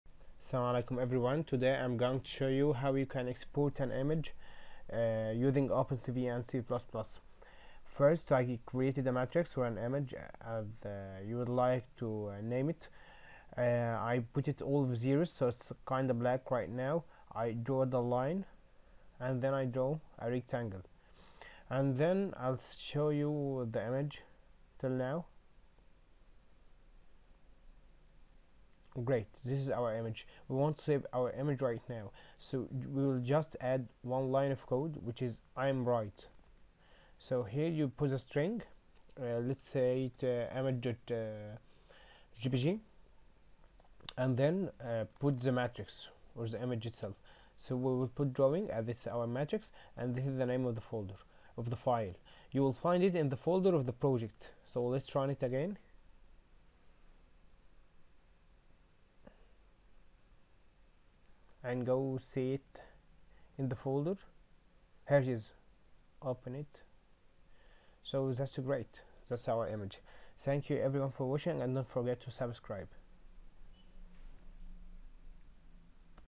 Lesson-11: Exporting -saving- an image using OpenCV and C++
Welcome to the 11th lesson in the OpenCV tutorials using c++
in this tutorial you will learn how to export your matrix to become an image .